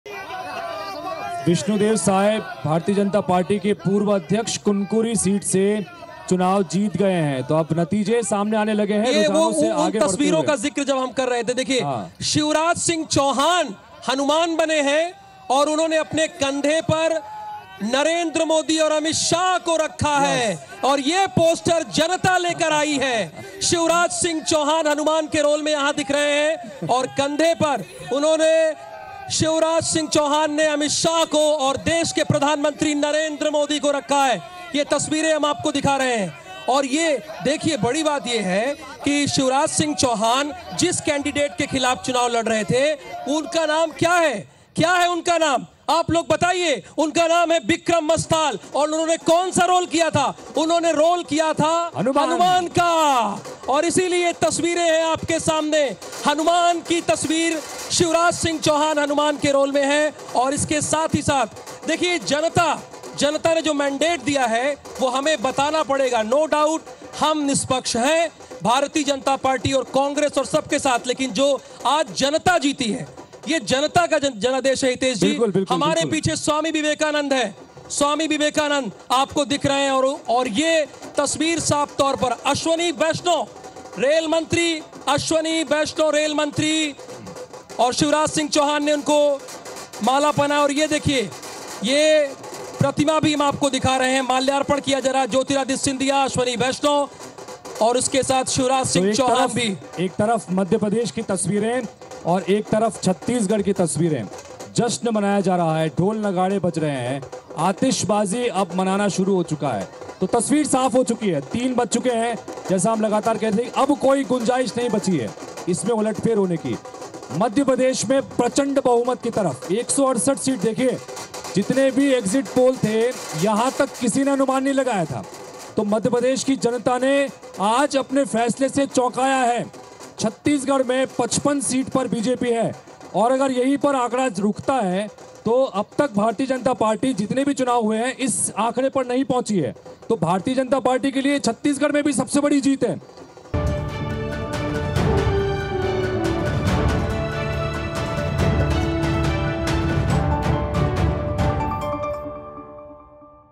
MP Assembly Election 2023: Madhya Pradesh में Shivraj बने हनुमान कंधे पर उठाया मोदी और अमित शाह को
Madhya Pradesh में Shivraj बने हनुमान कंधे पर उठाया मोदी और अमित शाह को